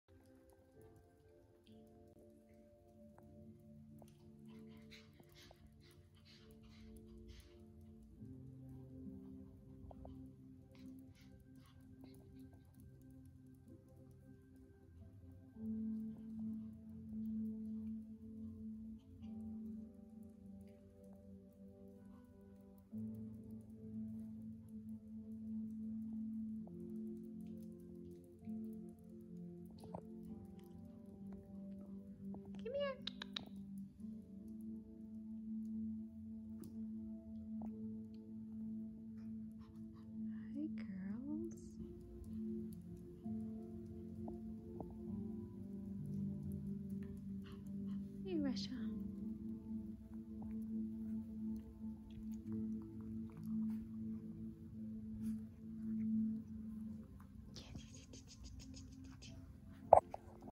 Russia and Kalisa Yorkies
Russia and Kalisa came to Lovebugs Rescue from a backyard breeder. The girls were in such terrible shape with matted coats, overgrown nails and horrible dental disease. This video shows how hoarder dogs behave when they are confined to small spaces most of their life. They would just walk in circles around the enclosed xpen. We are happy to report, both of these girls have been medical treated, groomed and have found forever homes. Once we acclimated them to our human world, the circling lessened over time.